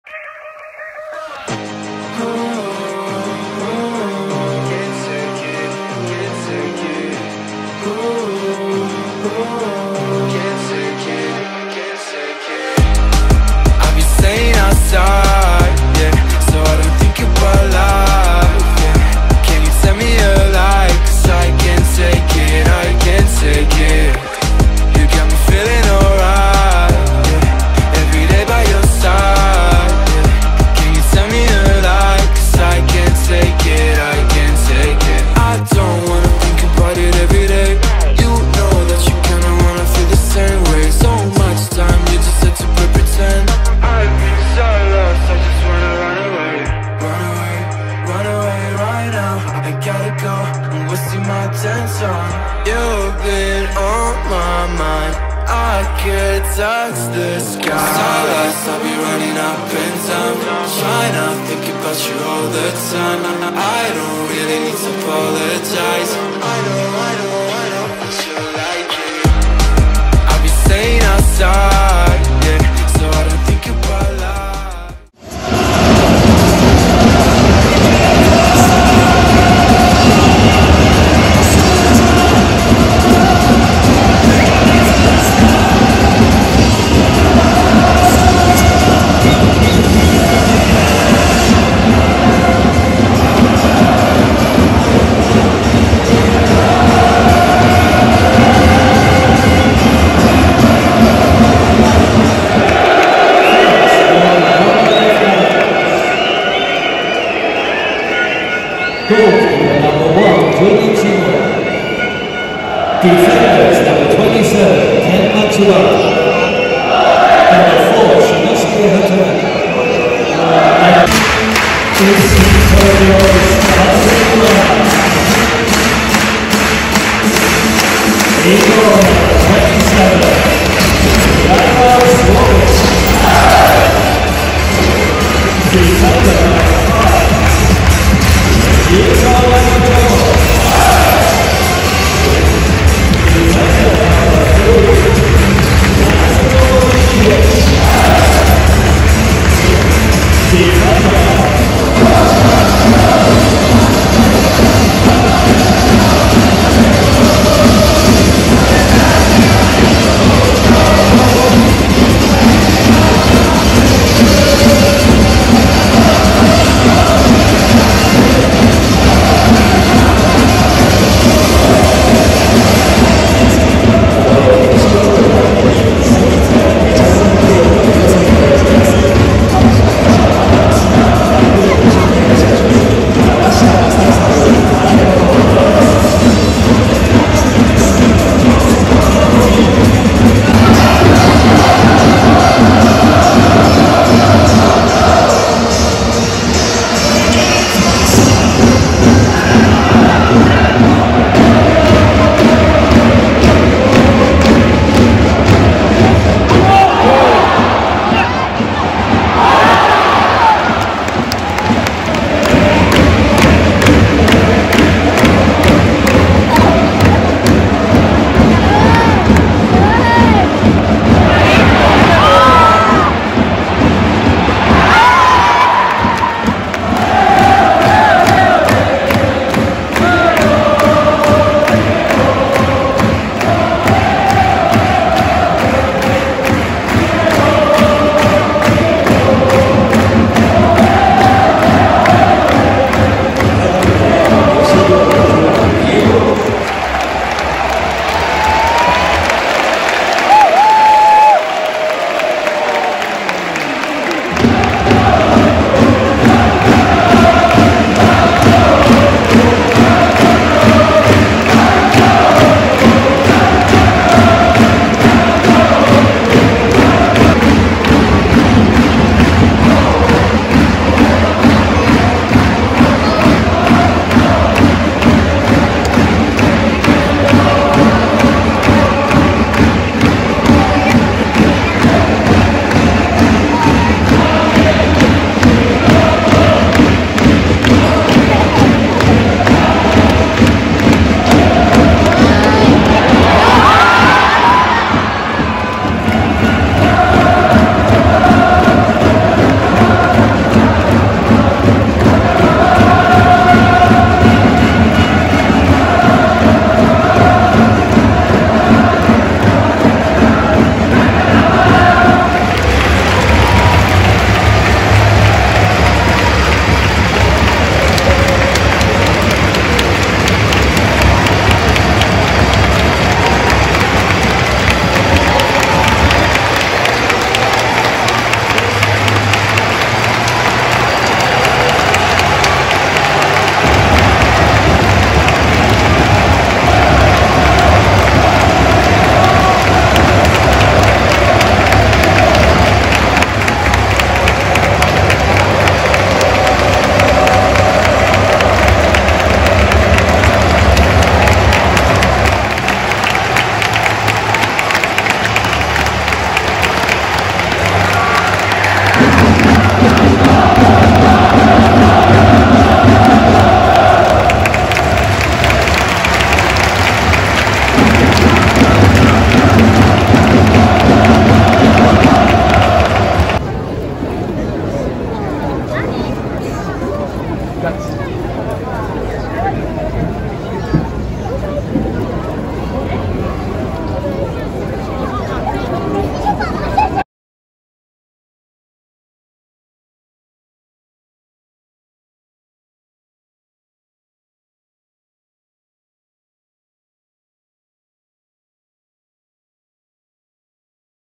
【J1リーグ】 FC東京 vs 横浜マリノス | FC Tokyo vs Yokohama Marinos AJINOMOTO Stadium
We went to the FC Tokyo vs Yokohama Marinos game at the Ajinomoto Stadium on 3rd June 2023. Unfortunately, FC Tokyo lost 3-2 but it was a great game! Both sets of fans were in full voice!

Check out our @WylieWestie channel for our other Japanese adventures with our cute Westie dog, Wylie :)